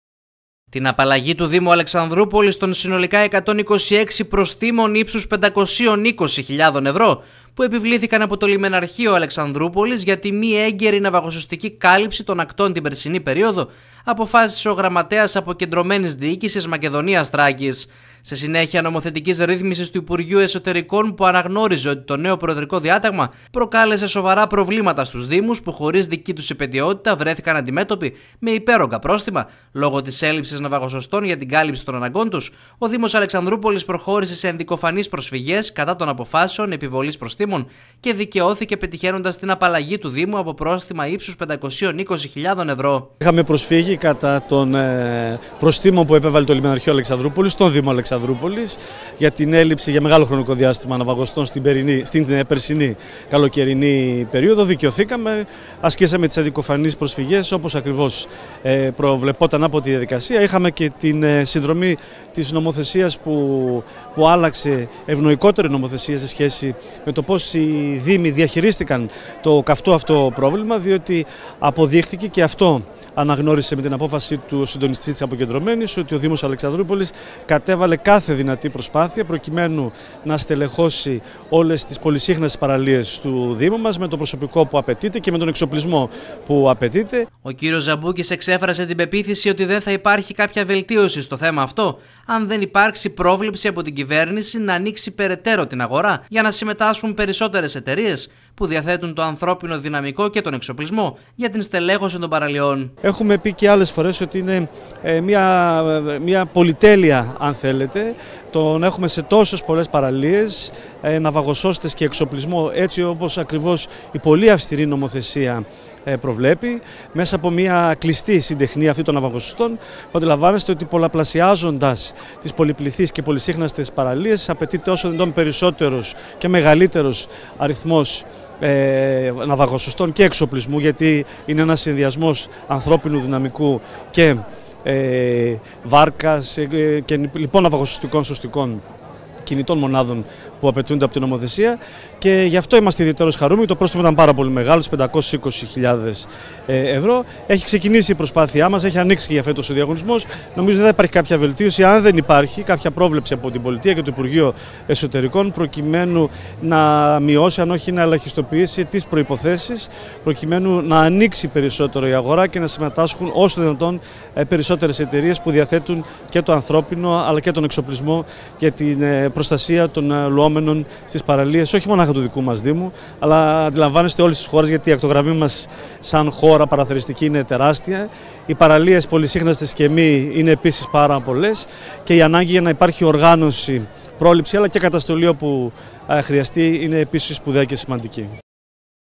Δικαίωση του Δήμου Αλεξανδρούπολης για τους ναυαγοσώστες - Απαλλάσσεται από πρόστιμα 520.000 ευρώ
Από πρόστιμα ύψους 520.000 ευρώ που του είχαν επιβληθεί για την μη έγκαιρη ναυαγοσωστική κάλυψη των ακτών την περσινή περίοδο, απαλλάχθηκε ο Δήμος Αλεξανδρούπολης. Αυτό γιατί όπως έκρινε ο Γραμματέας Αποκεντρωμένης Διοίκησης Μακεδονίας Θράκης, δεν υπήρχε υπαιτιότητα του Δήμου.